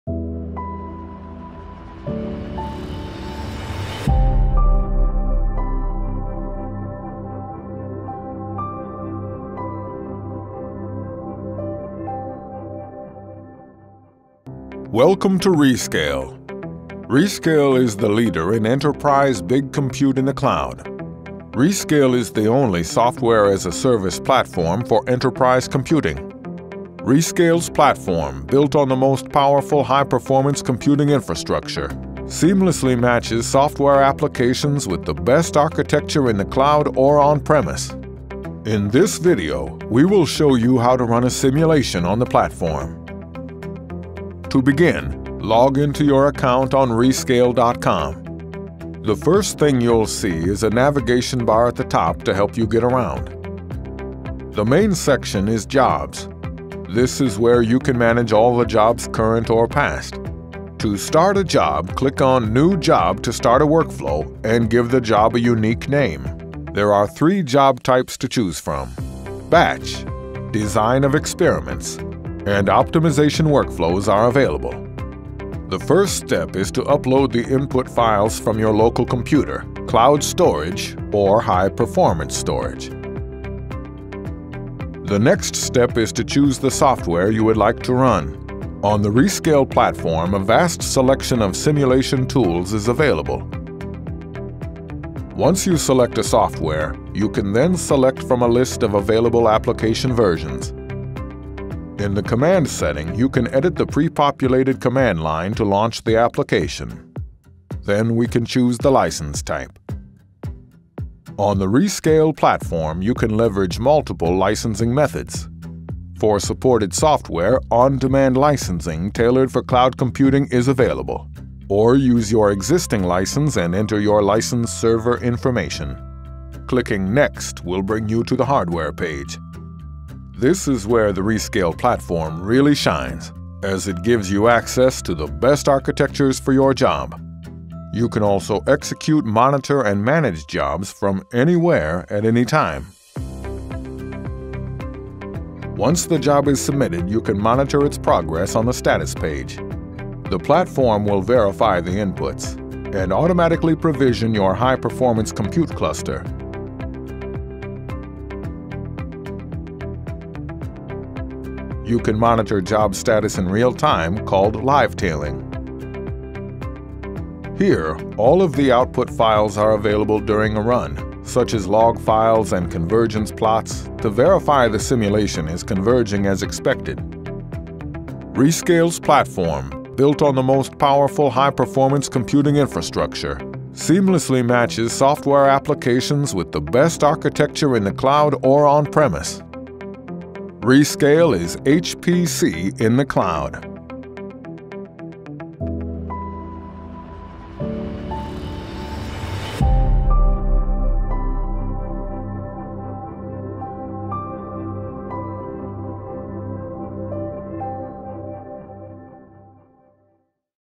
How to run an HPC job in the cloud | Rescale
Watch how simple a compute-intensive HPC job can be uploaded and run in the cloud using Rescale, the world's largest big compute platform.

In just a few clicks, you can access over 350 HPC applications (CFD, FEA, DL/ML or your own binary), and connect them to the world's most complete cloud HPC marketplace.